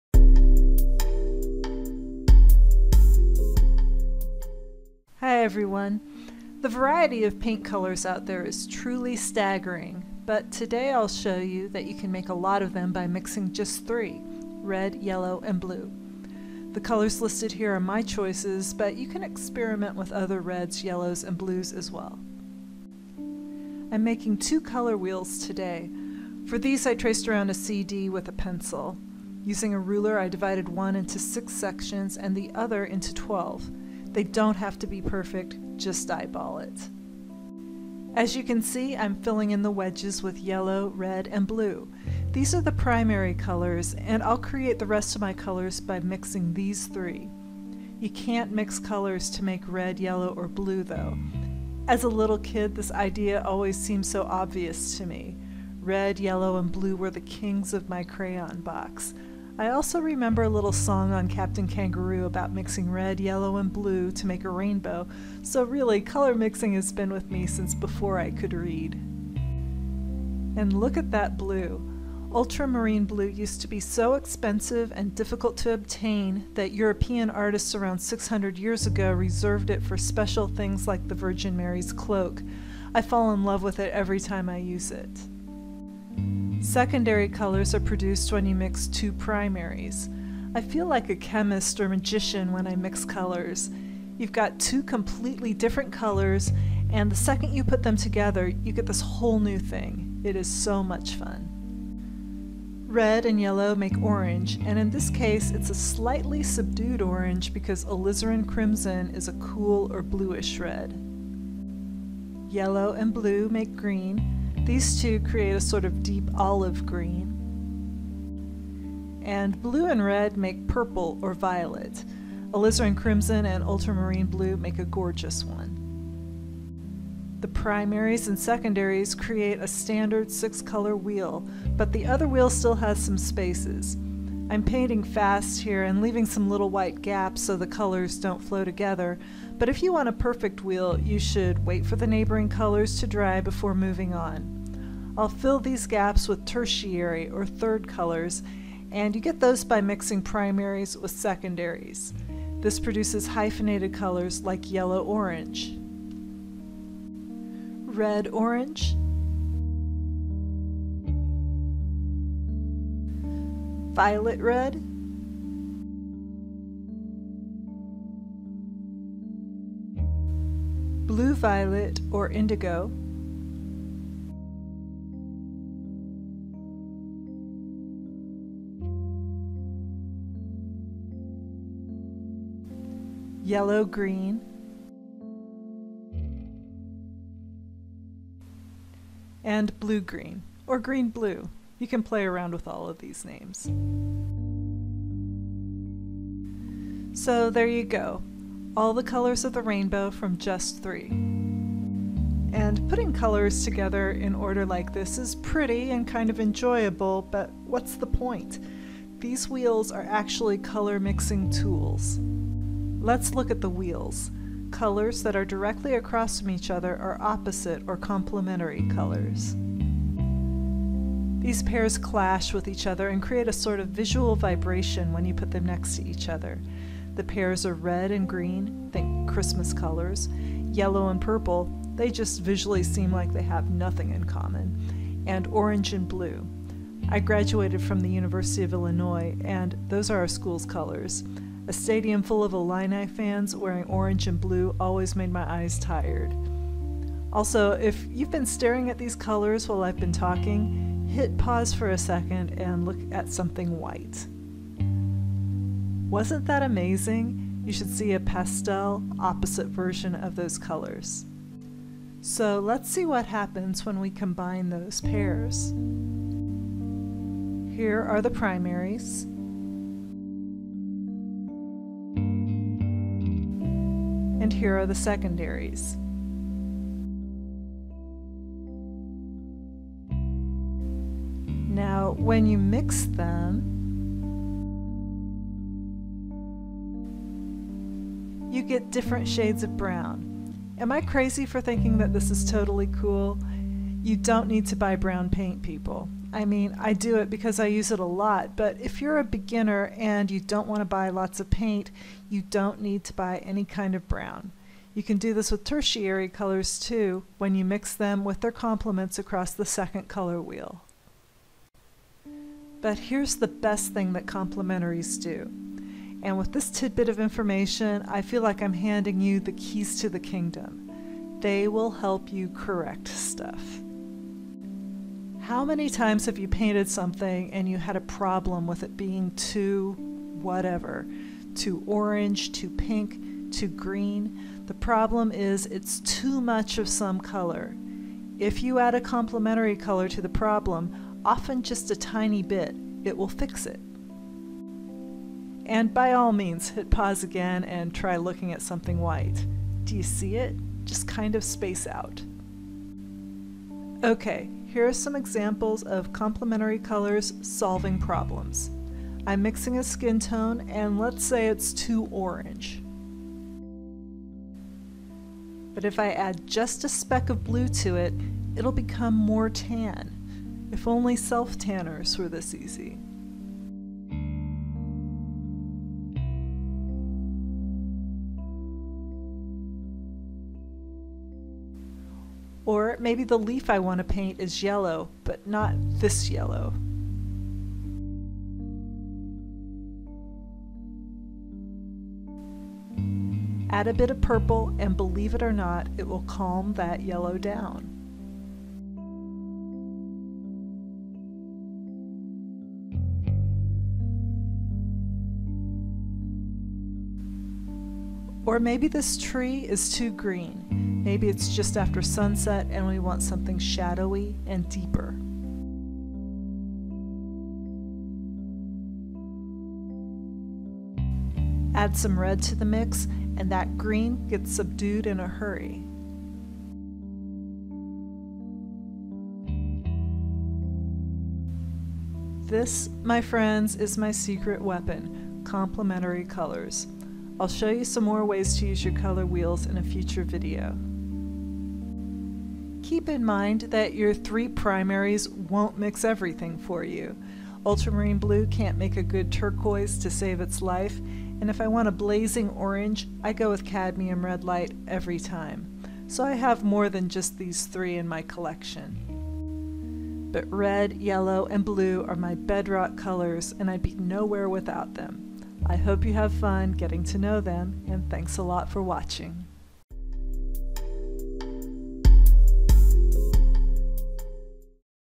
Color Wheel Tutorial - How To Mix Paint
Color wheels: they're not just pretty circular rainbows! They're also valuable tools for painters, and they can help you solve mixing problems. Learn about primary, secondary, and tertiary colors, plus discover how complementary colors can make you a better painter. This video also contains an optical illusion that may or may not work for you!

INSTAGRAM (new!): @kellyeddington

Colors used: alizarine crimson, cadmium yellow light, ultramarine blue, all by Old Holland

Paper: 9"x12" 140 lb. watercolor paper by The Langton (paper is attached to the rest of the block/pad, so no need for tape).

Brush: Yorkminster #5 round

Nail polish: Sinful Colors in Savage with Insta-Dri topcoat by Sally Hanson. I am also wearing a cheap mood ring that is turning my skin green.